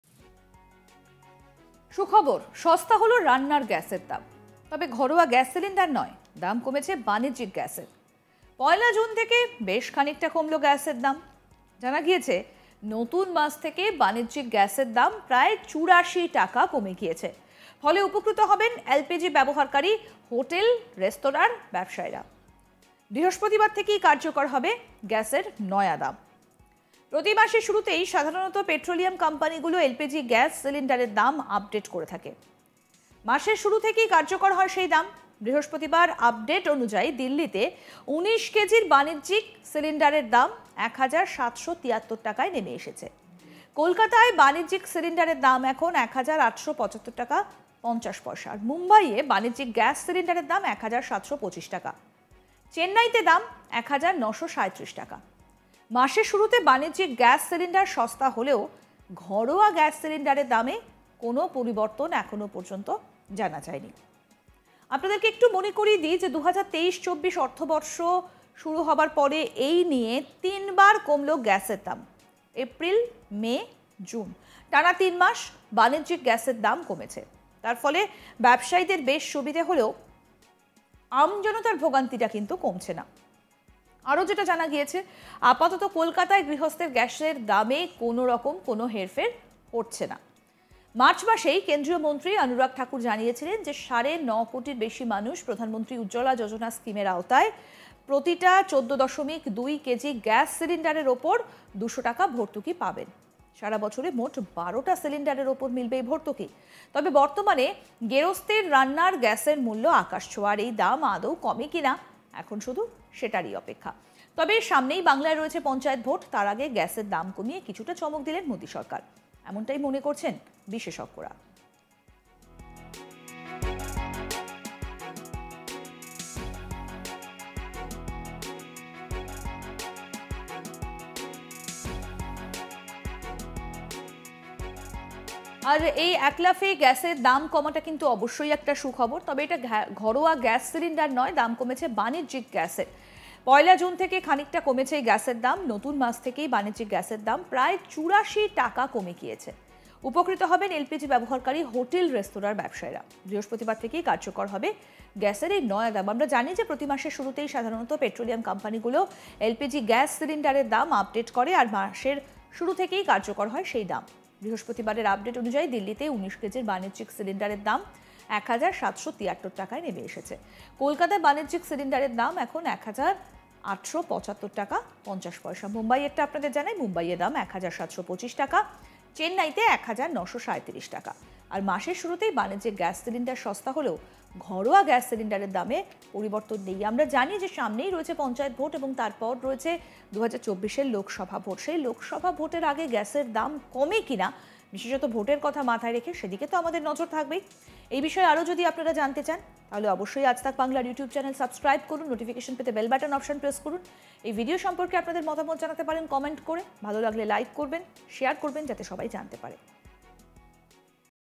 সুখবর, একলাফে অনেকটাই কমল গ্যাসের দাম | LPG Gas Price Today | Gas Subsidy | Aaj Tak Bangla
সস্তা হল রান্নার গ্যাসের দাম। তবে ঘরোয়া গ্যাস সিলিন্ডার নয়, দাম কমল বানিজ্যিক গ্যাসের।
1 জুন থেকে বেশ খানিকটা কমল গ্যাসের দাম। জানা গিয়েছে, নতুন মাস থেকে বাণিজ্যিক গ্যাসের দাম প্রায় চুড়াশি টাকা কমে গিয়েছে। ফলে উপকৃত হবেন এলপিজি ব্যবহারকারী হোটেল-রেস্তরাঁর ব্যবসায়ীরা। বৃহস্পতিবার থেকেই কার্যকর হবে গ্যাসের নয়া দাম।

প্রতি মাসের শুরুতেই সাধারণত পেট্রলিয়াম কোম্পানিগুলো এলপিজি গ্যাস সিলিন্ডারের দাম আপডেট করে থাকে। মাসের শুরু থেকেই কার্যকর হয় সেই দাম। বৃহস্পতিবার আপটেড অনুযায়ী, দিল্লিতে 19 কেজির বাণিজ্যিক সিলিন্ডারের দাম 1 হাজার 773 টাকায় নেমে এসেছে। কলকাতায় বাণিজ্যিক সিলিন্ডারের দাম এখন 1 হাজার 875 টাকা 50 পয়সা। মুম্বাইয়ে বাণিজ্যিক গ্যাস সিলিন্ডারের দাম 1 হাজার 725 টাকায় নেমে এসেছে। চেন্নাইতে দাম এখন 1 হাজার 937 টাকা।  মাসের শুরুতে বানিজ্যিক গ্যাস সিলিন্ডার সস্তা হলেও ঘরোয়া গ্যাস সিলিন্ডারের দামে কোনও পরিবর্তন নেই।

2023-24 অর্থবর্ষ শুরু হওয়ার পরে এই নিয়ে তিনবার কমল গ্যাসের দাম। এপ্রিল, মে, জুন- টানা তিনমাস বাণিজ্যিক গ্যাসের দাম কমেছে। তার ফলে ব্যবসায়ীদের বেশ সুবিধা হলেও আমজনতার ভোগান্তি কমছে না। জানা গিয়েছে, আপাতত কলকাতায় গৃহস্থের গ্যাসের দামে কোনও হেরফের করা হচ্ছে না।

মার্চ মাসেই কেন্দ্রীয় মন্ত্রী অনুরাগ ঠাকুর জানিয়েছিলেন, সাড়ে 9 কোটির বেশি মানুষ প্রধানমন্ত্রী উজ্জ্বলা যোজনা স্কিমের আওতায় প্রতিটি 14.2 কেজি গ্যাস সিলিন্ডারের উপর 200 টাকা ভরতুকি পাবেন। সারা বছর মোট 12টা সিলিন্ডারের উপর মিলবে এই ভরতুকি। তবে বর্তমানে গেরস্থের রান্নার গ্যাসের মূল্য আকাশছোঁয়া। এই দাম আদৌ কমে কি না, এখন তারই অপেক্ষা। তবে সামনেই বাংলায় পঞ্চায়েত ভোট। তার আগে গ্যাসের দাম কমিয়ে কিছুটা চমক দিলেন মোদি সরকার। এমনটাই মনে করছেন বিশেষজ্ঞরা।

Commercial LPG cylinder price slashed.